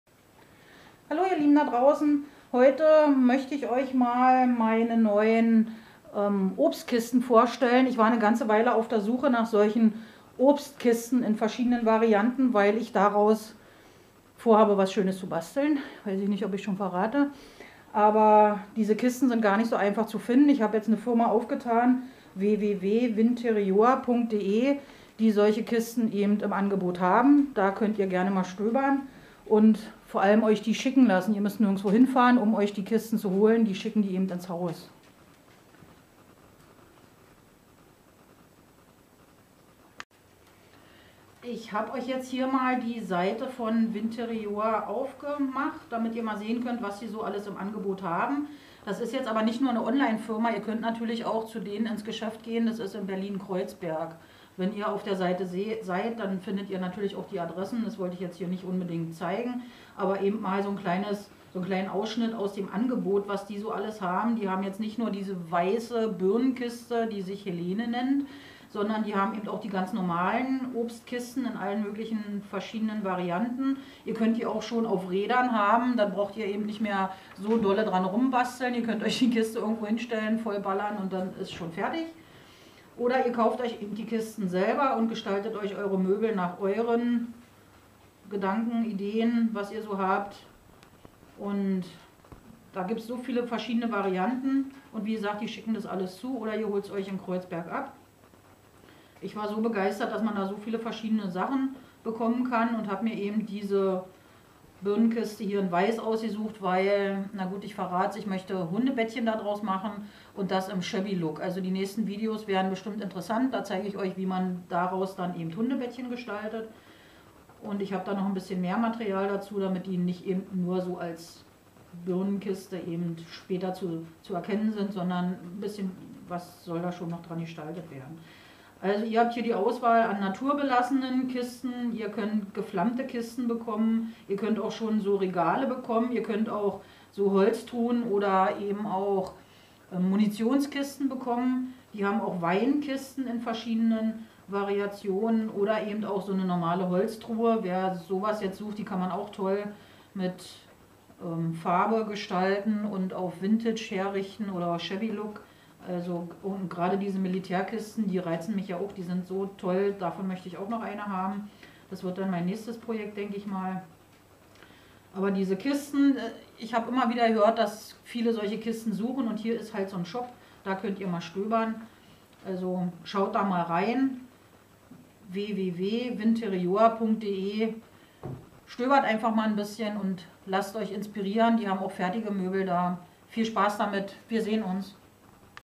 Obstkisten zum Basteln oder für Möbel von Vinterior
Hier zeigen wir euch, wo ihr Obstkisten bekommen könnt und wie die aussehen können. Ich nehme die gern zum Basteln und gestalte da kleine Möbel draus.